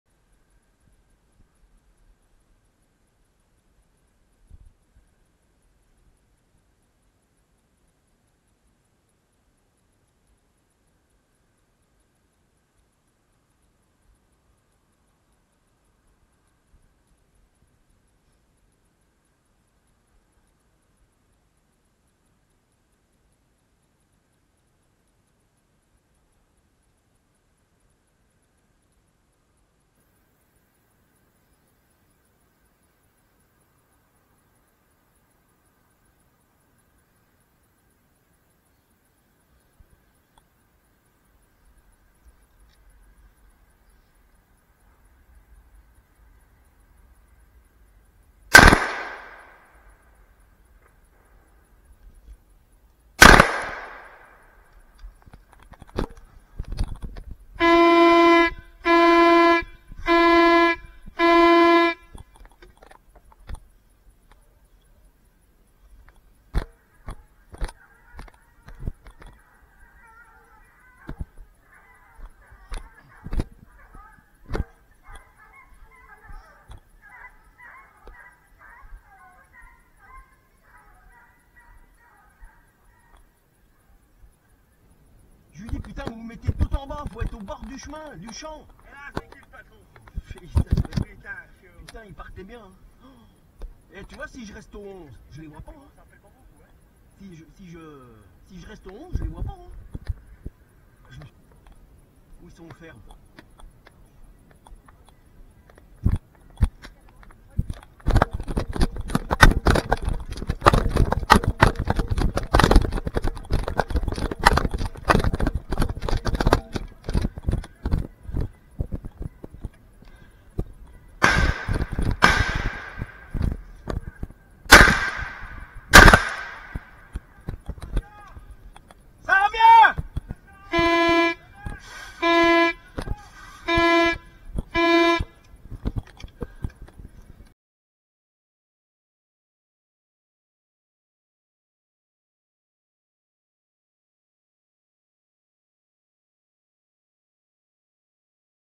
chasse aux sangliers dans le Jura(39): 4sangliers au tableau
Les sangliers sont remis.J'ai fait 2 lignes de postés en limite derriere et en limite devant.Je suis seul avec les 2 traqueurs et vu la configuration du terrain je peux tirer sans risque sur ma droite et sur ma gauche.